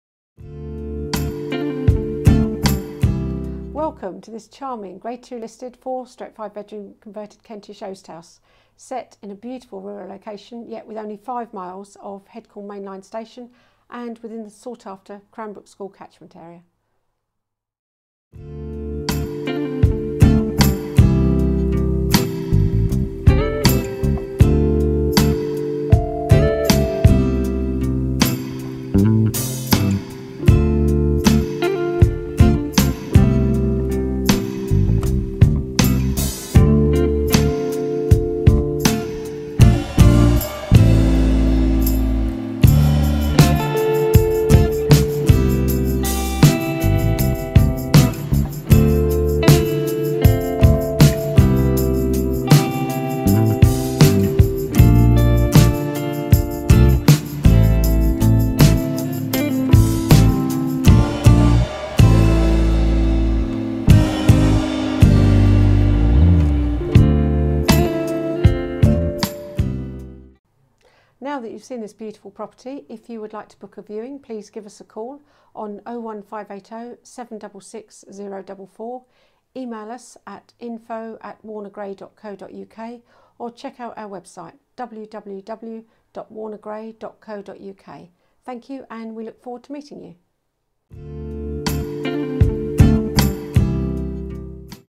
Frittenden Road, Biddenden. WarnerGray Estate Agents
A beautiful Grade II listed 4/5 Bedroom property forming part of an impressive Kentish oast full of character, set with good size gardens adjoining open fields to rear and off road parking.  Country lane location and falling in the Cranbrook School catchment area.